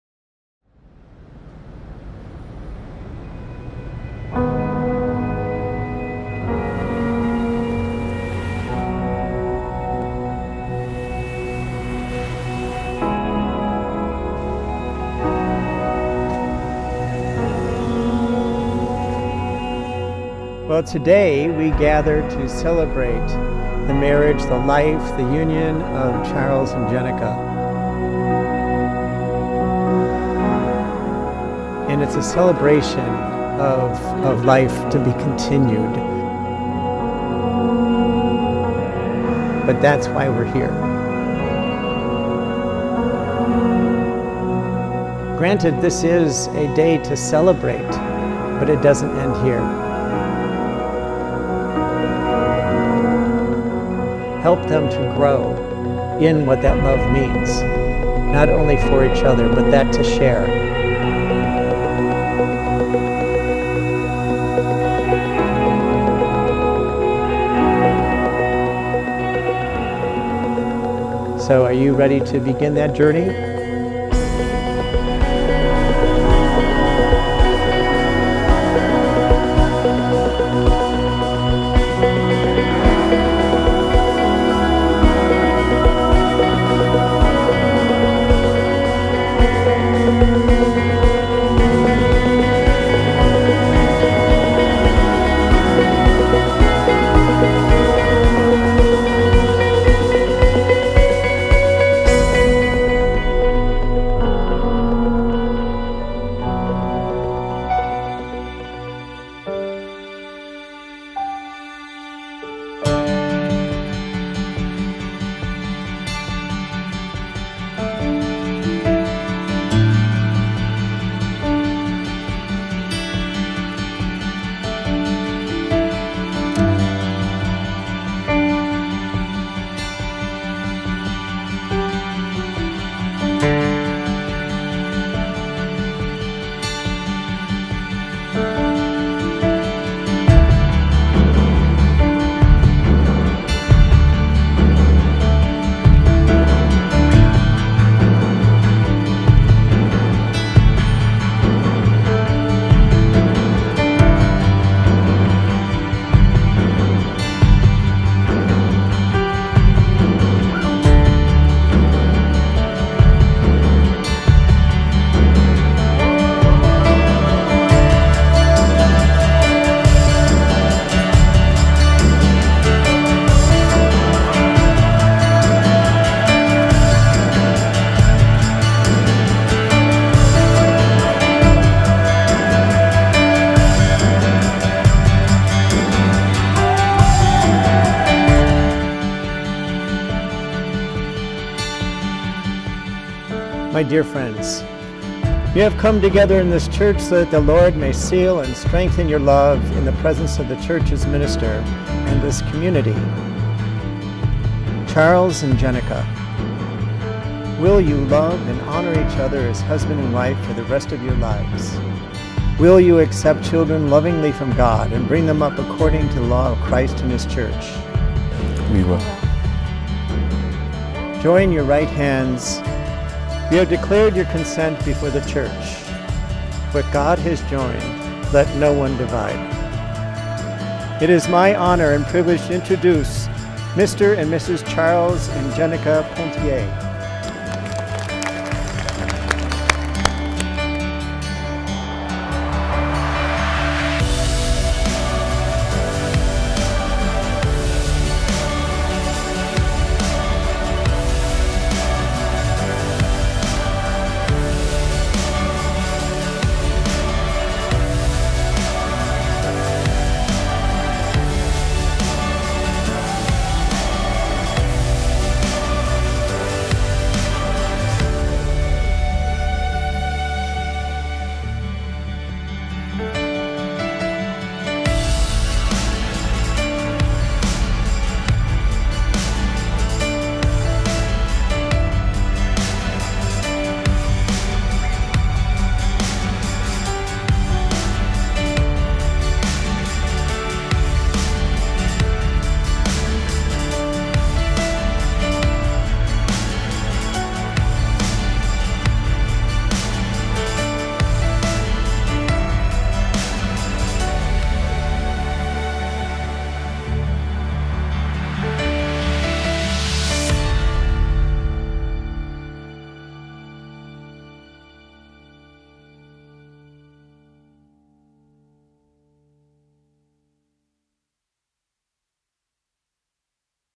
Jenica & Charles (Highlight)
An exquisite San Francisco wedding covering iconic SF locations from a historic church to the Embarcadero.